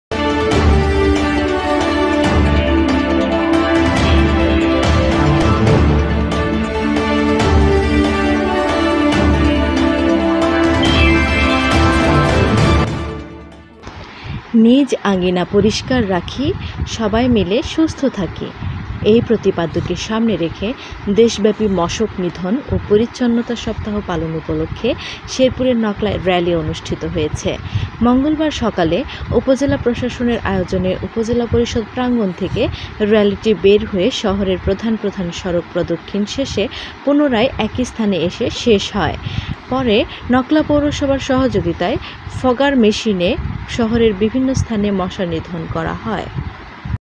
নকলায় মশক নিধন ও পরিচ্ছন্নতা সপ্তাহ উপলক্ষে শোভাযাত্রা-30-7-19
নকলায় মশক নিধন ও পরিচ্ছন্নতা সপ্তাহ উপলক্ষে শোভাযাত্রা